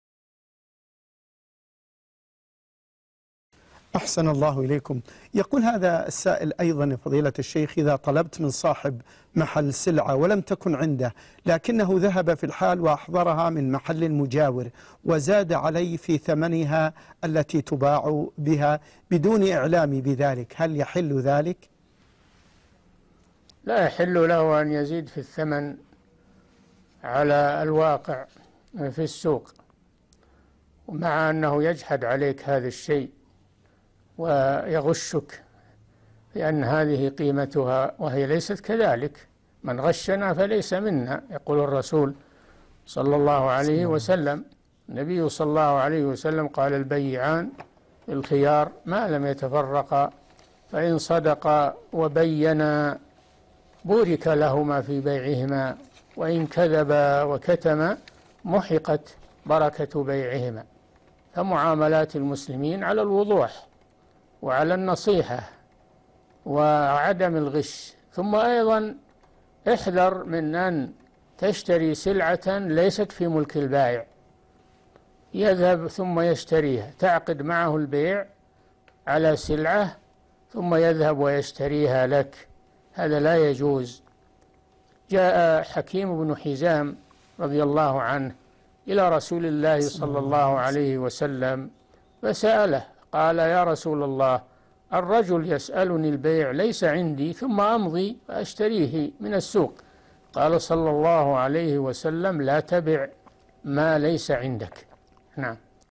طلبت سلعة من صاحب المحل فأحضرها من المحل المجاور وباعها لي، فما حكم البيع؟ الشيخ صالح الفوزان
نص السؤال: إذا طلبت من صاحب محل سلعة ولم تكن عنده لٰكِنَّه ذهب في الحال وأحضرها من محل مجاور وزاد عليه في ثمنها الَّتي تباع بها بدون إعلامي بذلك، هل يحل ذَ ٰلِكَ؟ رابط الحلقة كاملة
السيرة الذاتية :
للاشتراك في قناة الشيخ صالح الفوزان
لمشاهدة حلقات فتاوى على الهواء كاملة

رئيس الفريق
لمشروع كبار العلماء